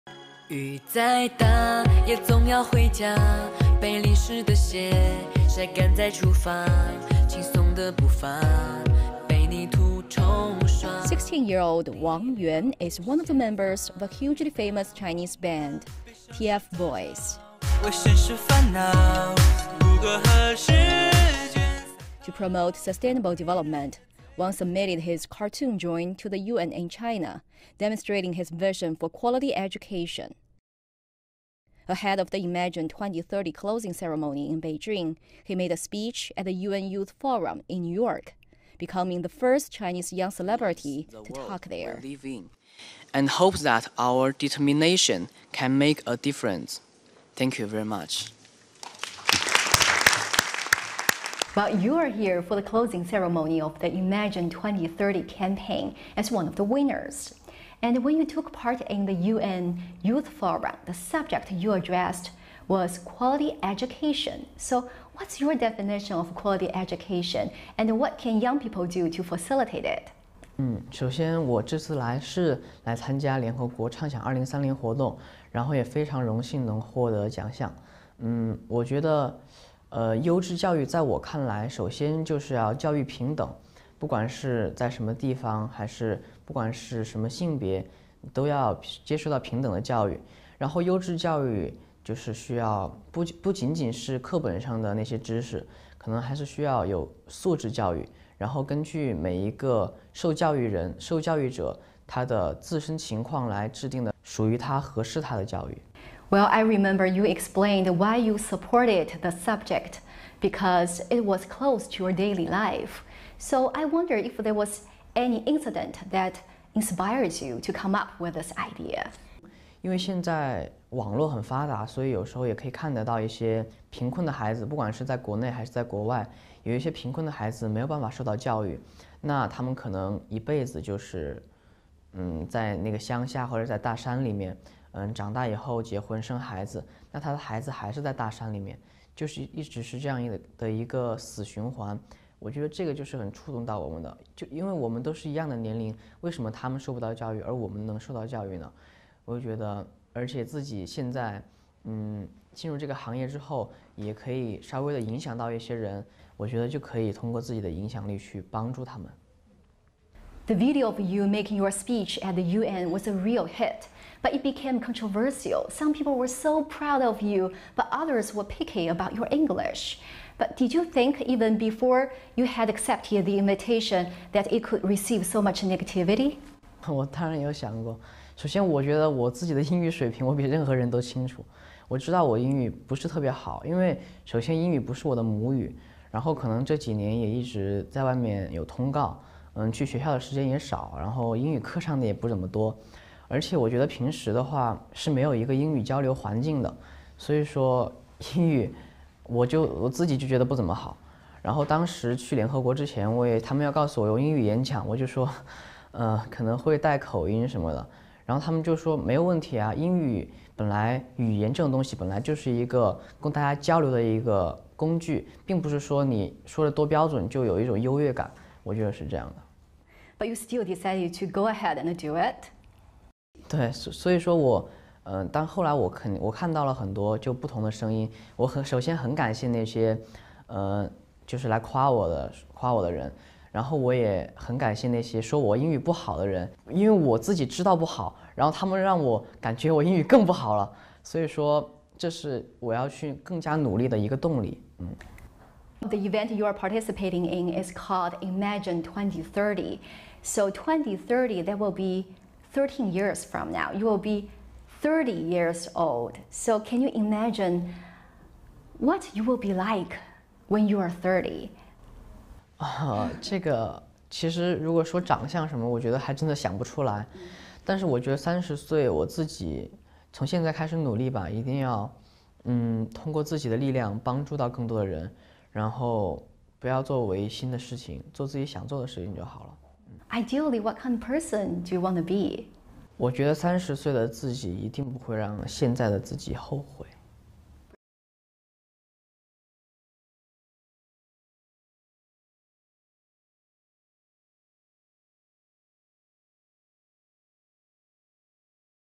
Pop star Wang Yuan of TFBOYS shares his views of the world in 2030
Seventeen young people in China were recognized for their creativity in the United Nations' "Imagine 2030 Campaign" in China. The youth had all expressed their visions of the world in 2030 if one of the U.N.'s 17 development goals was achieved. They used painting, video, and other forms. Pop singer and idol Wang Yuan, from famous Chinese band TFBOYS, also participated in the event. CGTN Reporter, Wang Hui, sat down with Wang Yuan, to talk about his experience with the campaign and more.